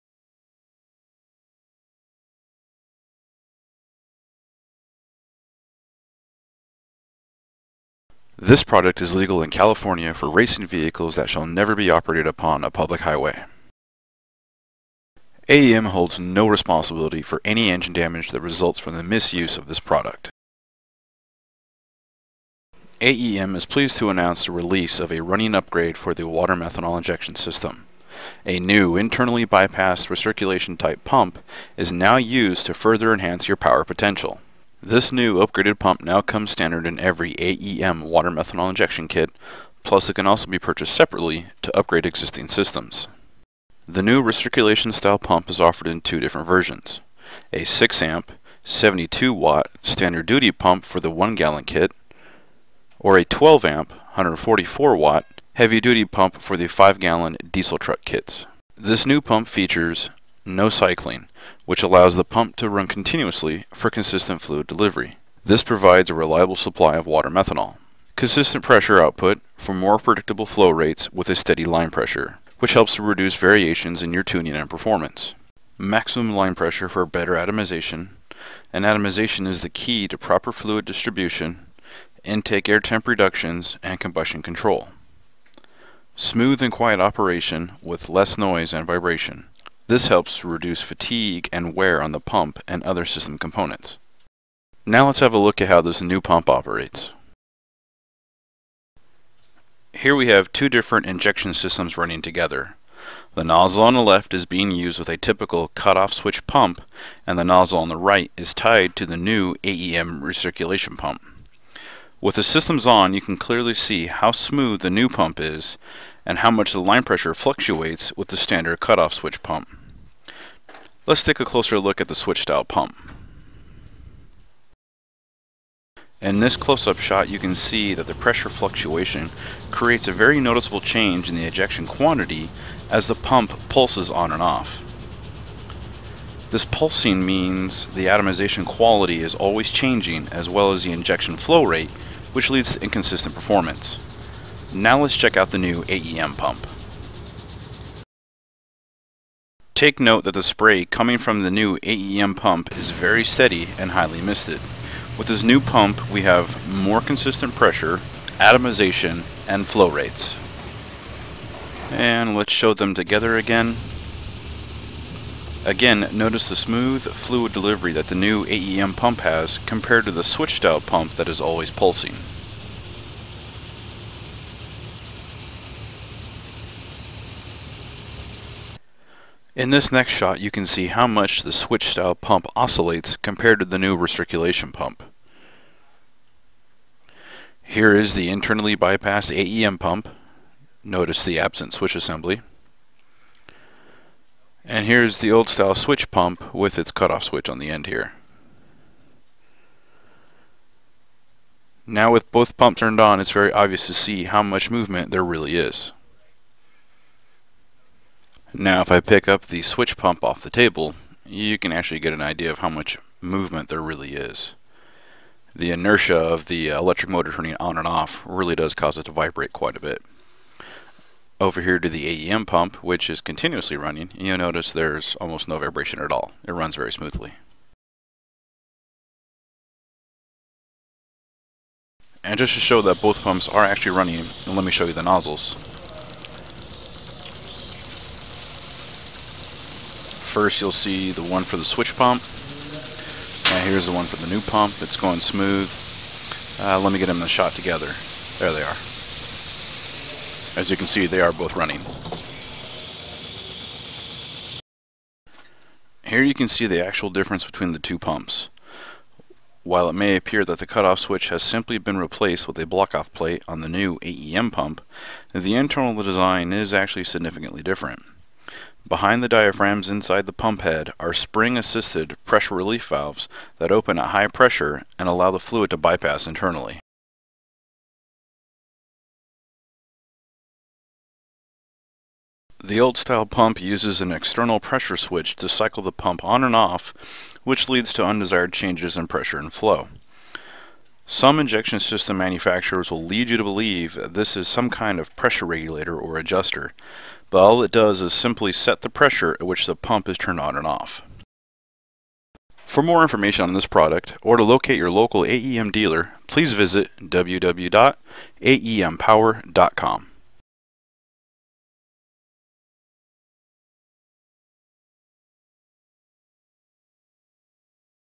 AEM Water/Methanol Injection Kits
These AEM Power water/methanol injection kits allow you to increase boost pressure and advance timing without having to run higher octane fuel. They include either a 1-gallon or 5-gallon reservoir with a fluid level sensor already installed. Also included is AEM Power's Boost Safe output; this allows you to retard timing or reduce boost if the systems run out of fluid or detect an error. Once installed, the AEM water/methanol injection kits use a boost-dependent, variable-flow, progressive controller to determine the amount of water/methanol mixture to feed into the inlet stream from the reservoir. Adjustments can be made with the three included jets, resulting in significant horsepower gains.